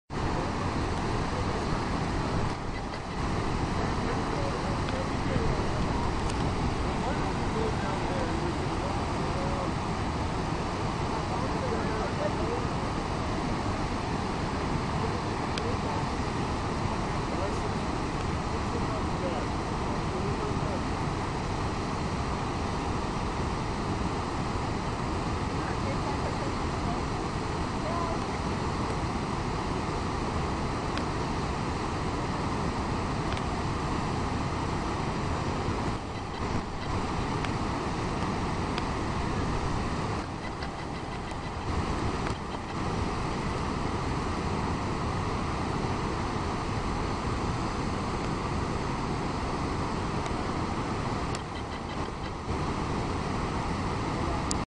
Cameron Falls At Waterton Lakes National Park July 2008
Looking at Cameron Falls at the Waterton Lakes National Park. This waterfall flows eventually into Waterton Lake. Find it just off of from the main townsite right in the heart of the park.

Fun Fact: Sometimes sediment from Red Rock Canyon turns this waterfall a reddish, pinkish colour after a heavy rainfall. Rare but impressive sight to see if you or I get to see it.